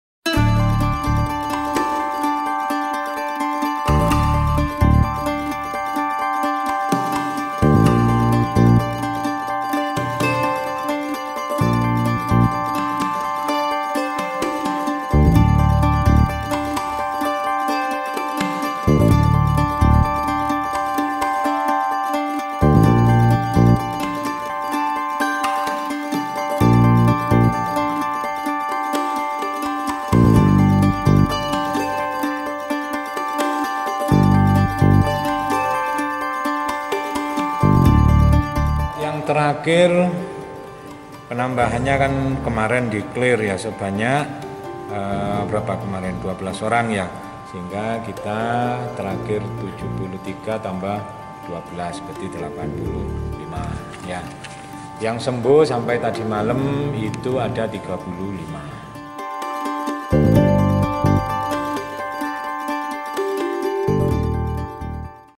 Tambahan 12 Pasien Positif Corona, Separuhnya Dari Klaster Keluarga
MALANG-KOMPAS.TV-Di masa transisi new normal, jumlah penambahan pasien terkonfirmasi positif covid-19 terus bertambah.

Kamis (11/06/2020) jumlah pasien positif corona di Kota Malang bertambah 12 orang, sehingga total pasien positif corona ada 85 orang.

Dari penambahan 12 orang tersebur, 6 diantaranya berasal dari klaster keluarga.

Kasus tersebut berawal dari satu orang kemudian menularkan ke anggota keluarga yang lainnya.

Selain dari klaster keluarga, pasien positif corona juga berasal dari tenaga kesehatan.

Menurut Juru Bicara Satgas Covid 19 Kota Malang Husnul Muarif, adanya penambahan pasien positif corona yang cukup signifikan ini karena hasil tes yang keluar secara bersamaan.

Husnul menanbahkan, setiap petugas kesehatan dari Dinkes Kota Malang juga melokalisir pasien yang terkonfirmasi positif.

Selain itu petugas juga melakukan pelacakan titik yang berpotensi untuk dilakukan rapid test atau tes cepat.